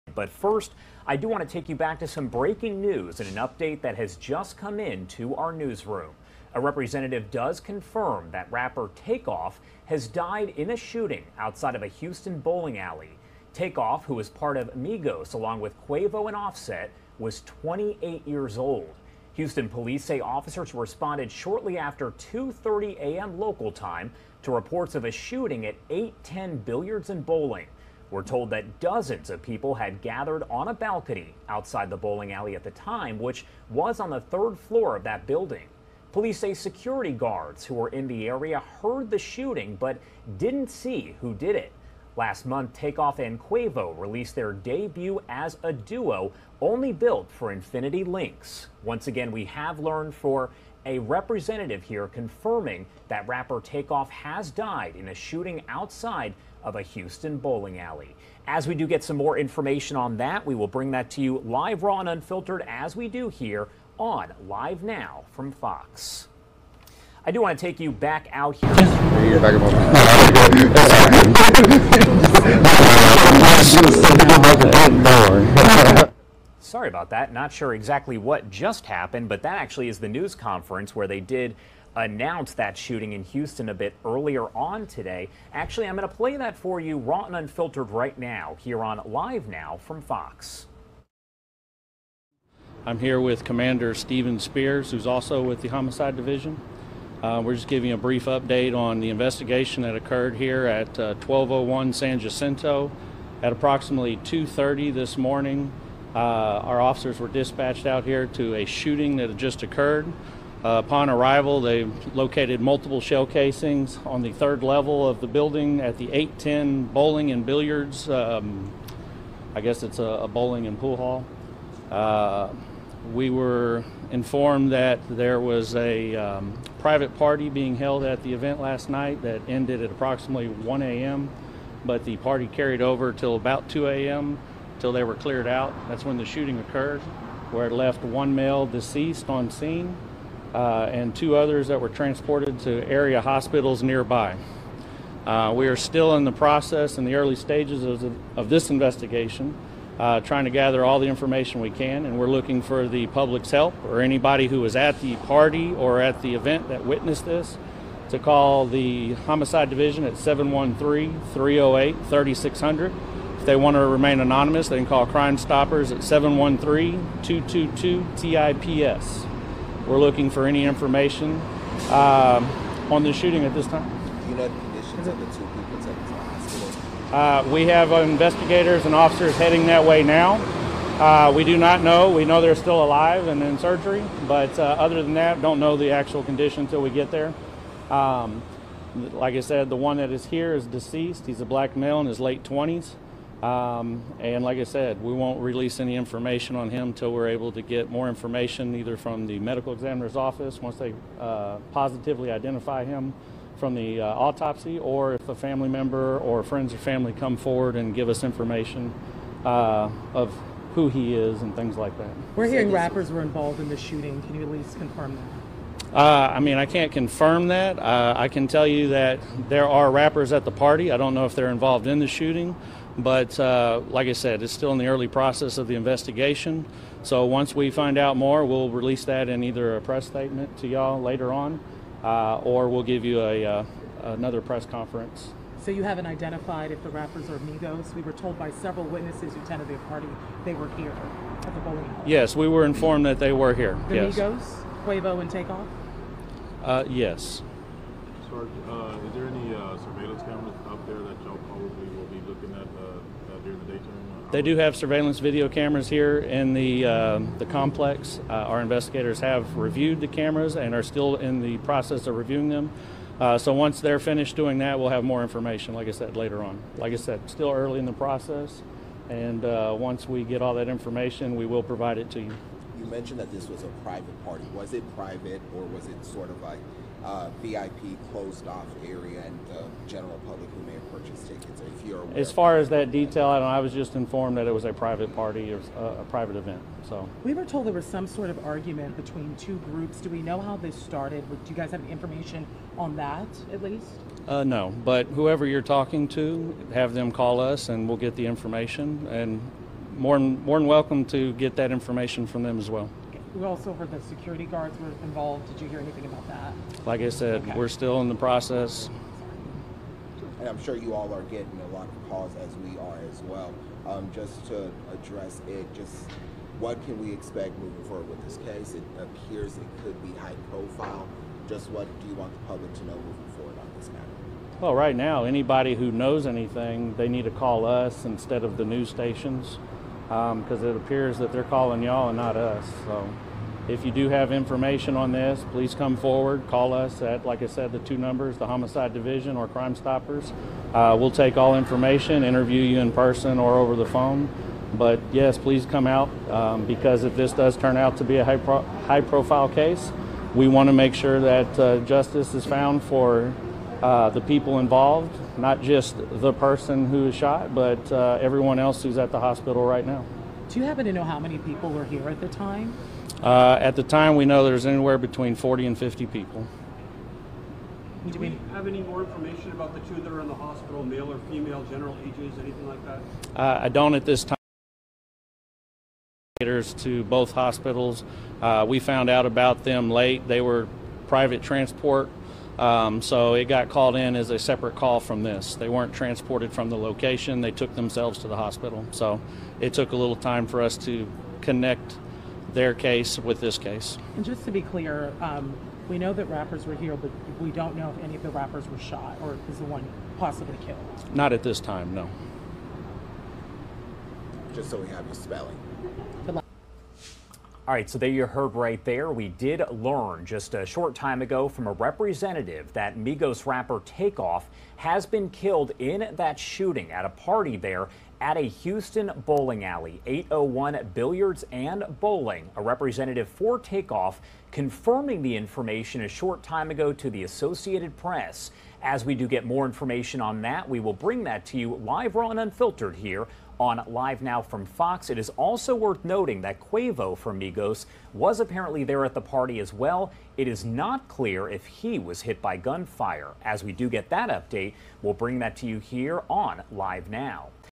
Migos rapper Takeoff killed in bowling alley shooting: New details just released | LiveNOW from FOX
Takeoff, a member of the rap group Migos, was shot and killed at a bowling alley in Houston overnight, representatives for the rapper confirmed. Police have not identified any suspects in the shooting.